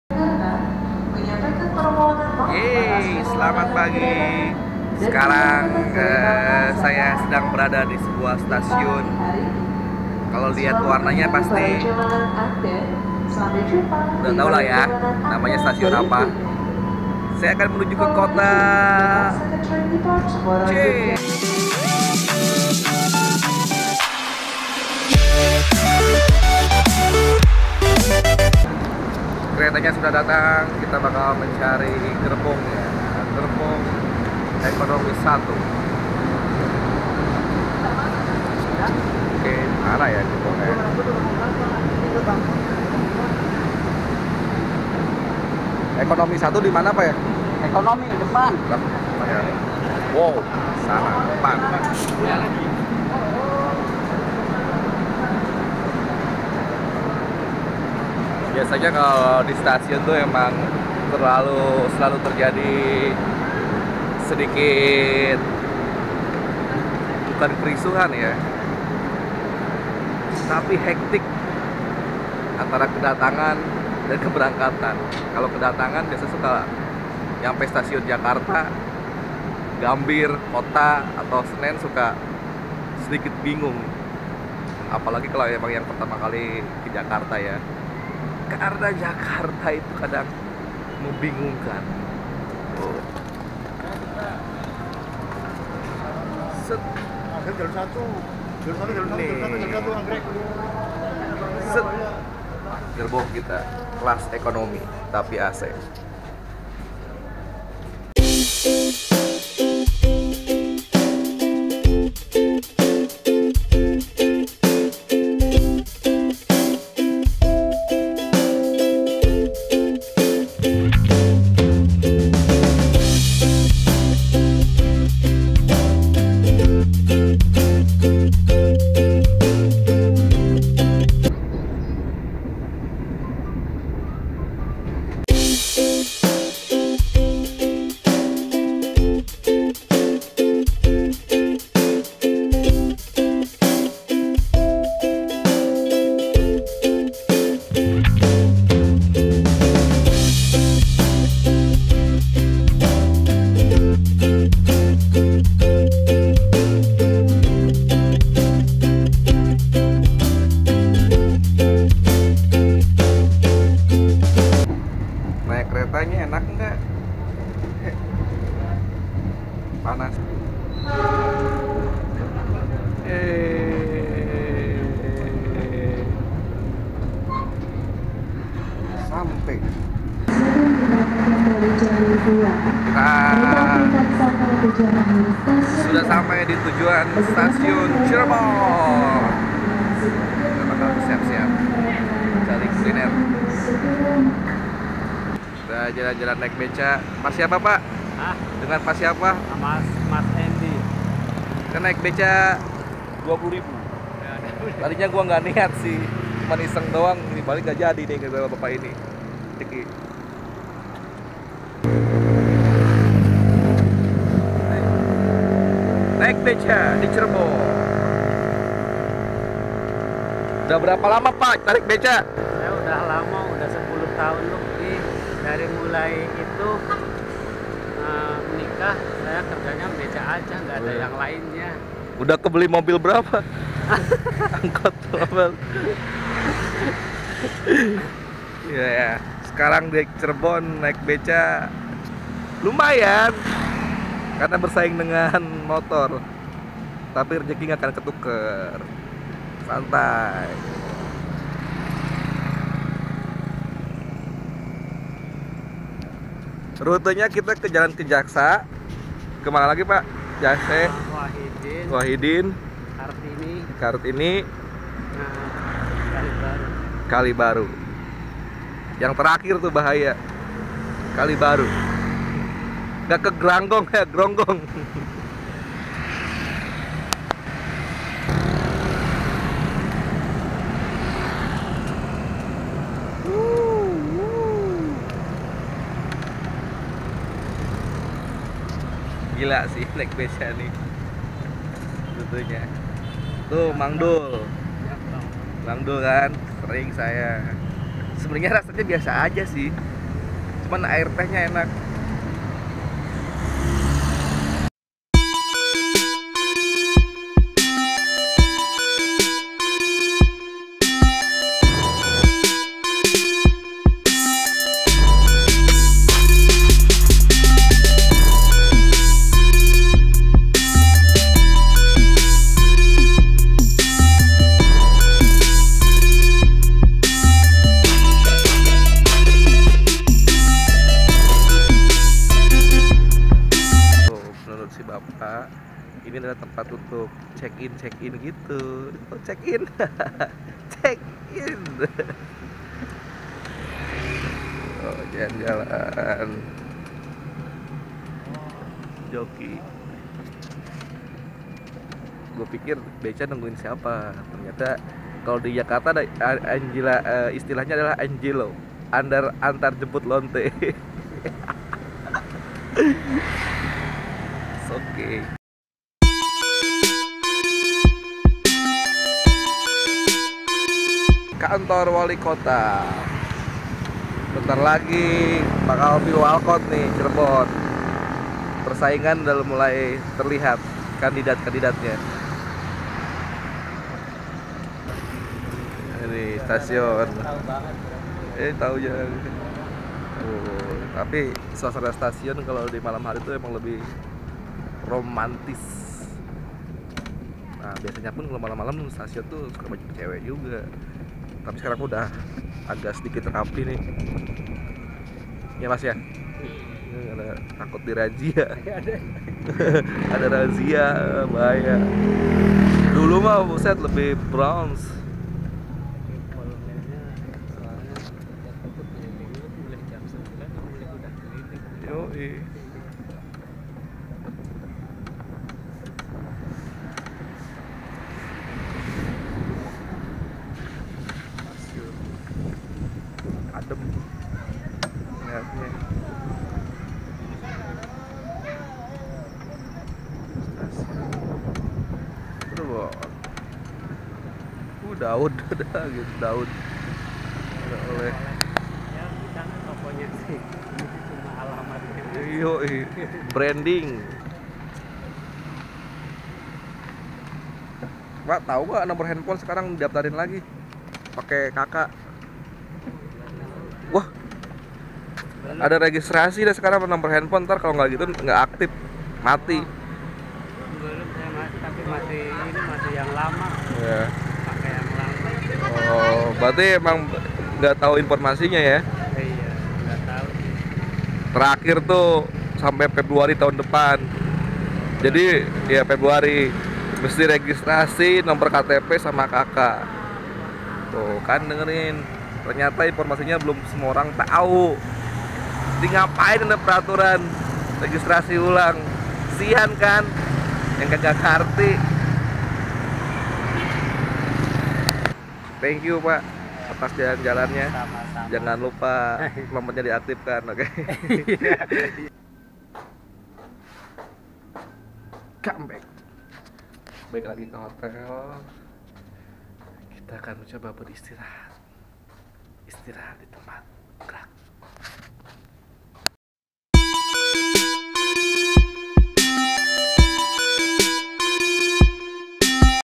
Naik Kereta & Becak Mlm hari (Cirebon Bag 1) CeritaSiTipot EPS 23
Di EPS 23
Seru-seruan sebentar Ke Cirebon Naik Kereta Api dari Stasiun Gambir lalu keliling kota naik becak sekaligus melakukan Interview Exlusif bersama Mas Endi sebagai tukang becak Profesional, dan ternyata sensasi naik becak malam-malam di Cirebon itu cukup mengasikan sekali loh..

Noted : Konten Vlog ini tanggung Jawab Pribadi yah